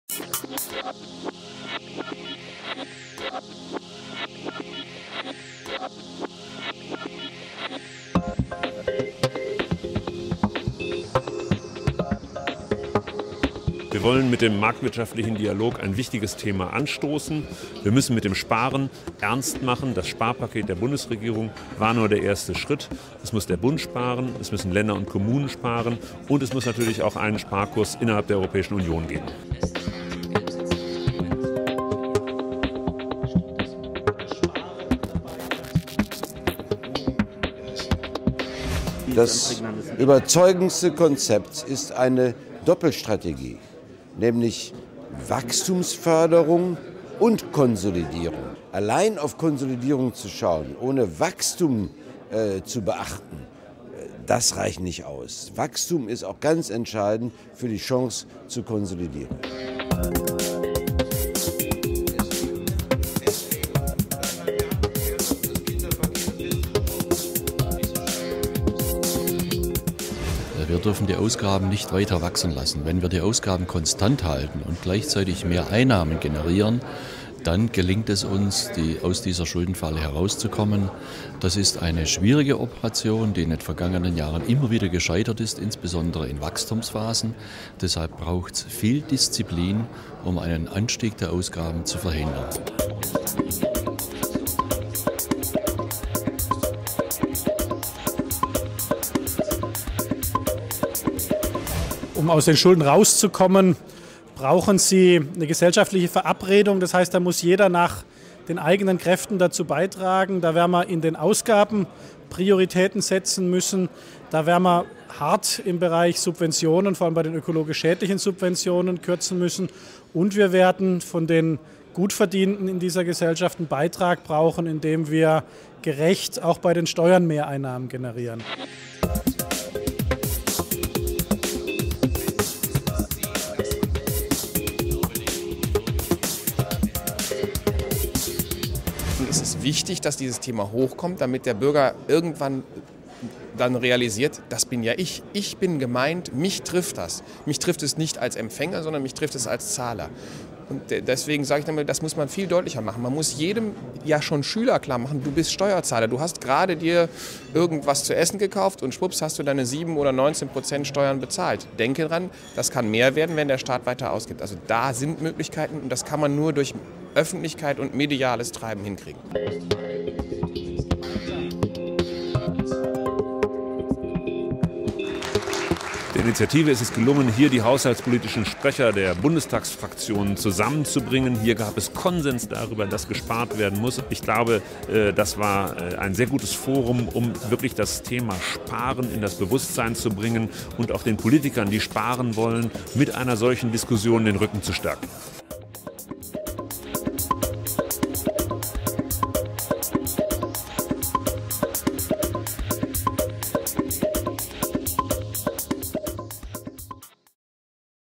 Marktwirtschaftlicher Dialog "Raus aus der Schuldenfalle"
Die Haushaltsexperten der Bundestagsfraktionen von Union, SPD, FDP und Grünen haben auf einer Veranstaltung der Initiative Neue Soziale Marktwirtschaft (INSM) in Berlin ihren Willen zur Haushaltskonsolidierung bekräftig. Norbert Barthle (CDU/CSU), Joachim Poß (SPD), Otto Fricke (FDP) und Alexander Bonde (Grüne) fordern, die Vorgaben des Grundgesetzes und der Schuldenbremse einzuhalten. Sowohl der Bund wie auch die Bundesländer müssten ihre Neuverschuldung konsequent zurückführen.